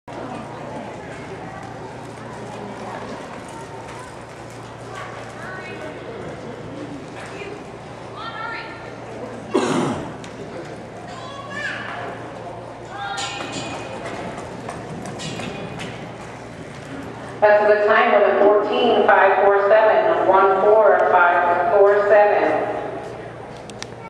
Guy and Yvonne, Angel's benefit, Trenton 4-3-16
Guy ran a 14.547, with a 14.164 winning it. 209 horses. Nice and smooth!